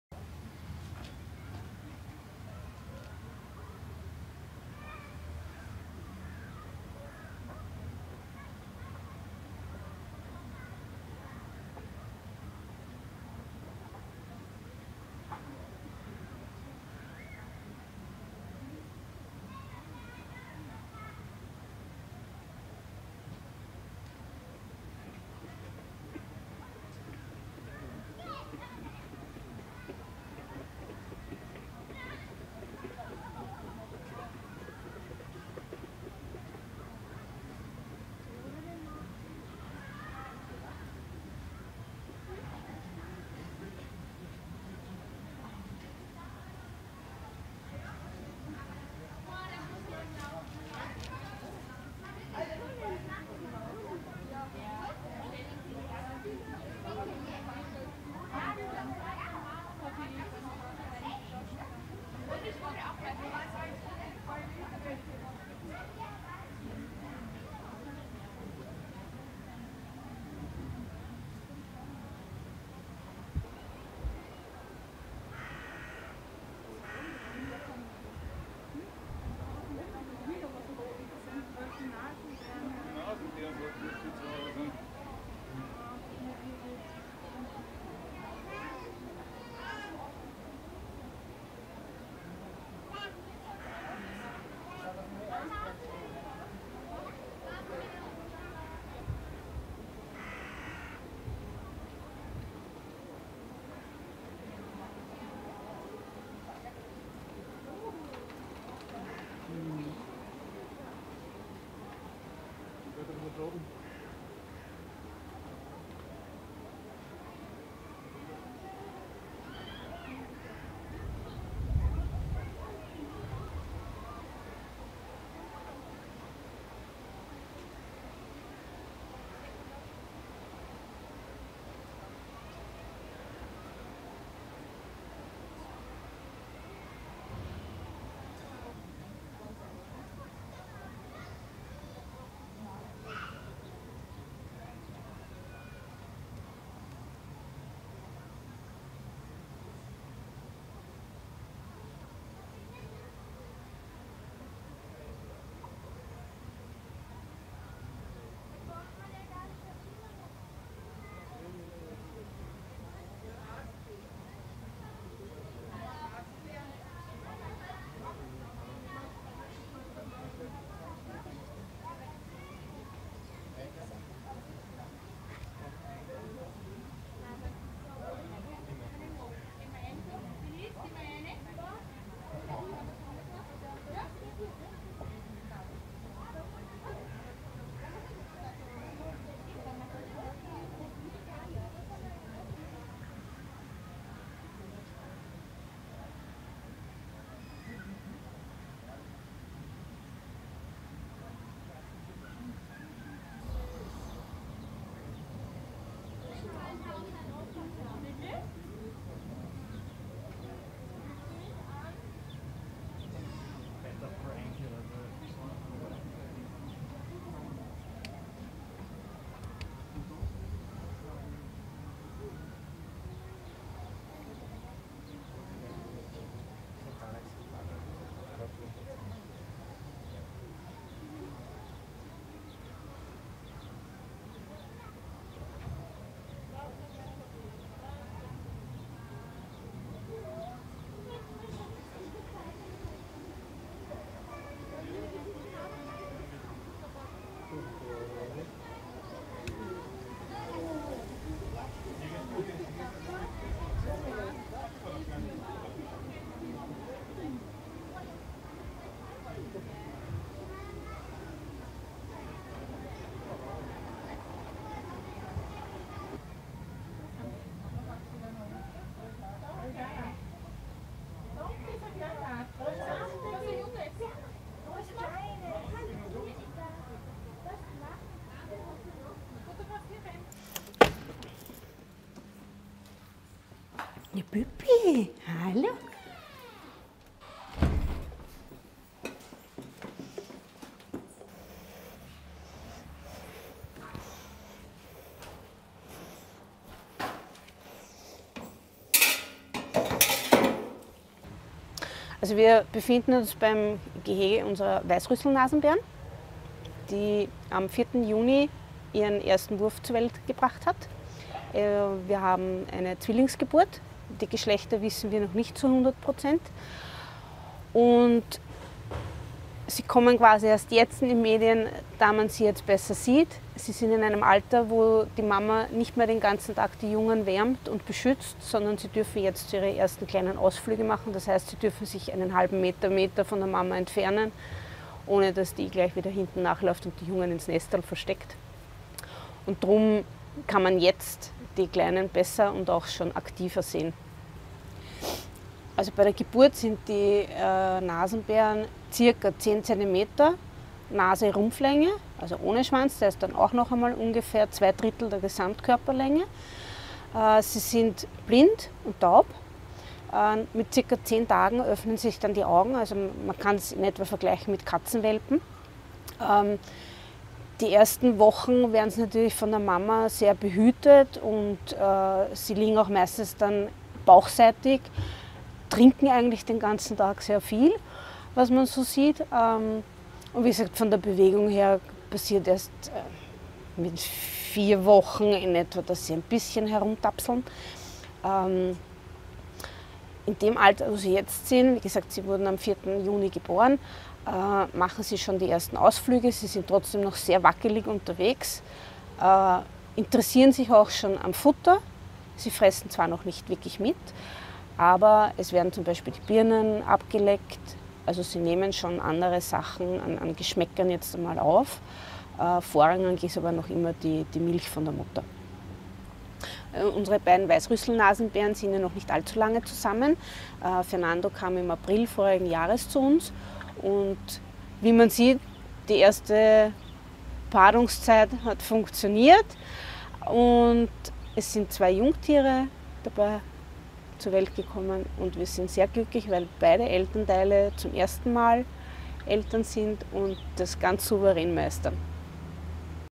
Adorable Baby Coatis Twins Born At Worlds Oldest Zoo
These are the adorable baby white-nosed coati twins that have been born at the Schoenbrunn zoo in the Austrian capital Vienna.

The self-styled "world's oldest zoo" welcomed two adorable newborn twins on 4th June after mother Puppe gave birth to the babies, whose sex is not yet known as they are still suckling.